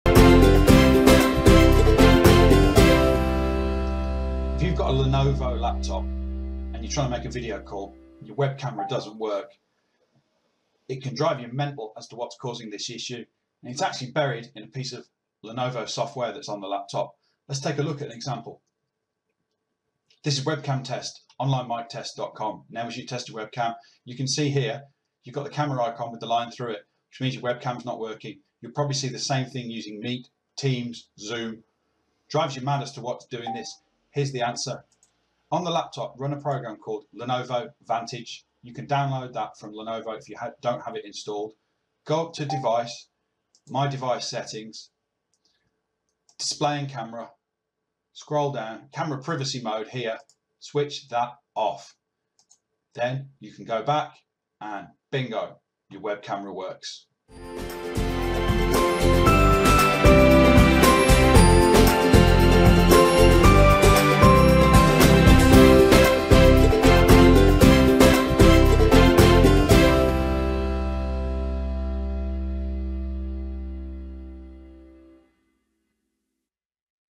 How to get your webcam working on your Lenovo laptop.
Lenovo laptops have a hidden webcam privacy setting in the pre-installed Lenovo advantage software, you need to toggle this setting, and then your webcam will start working in video calling apps such as Google Meet, Zoom and Microsoft Teams.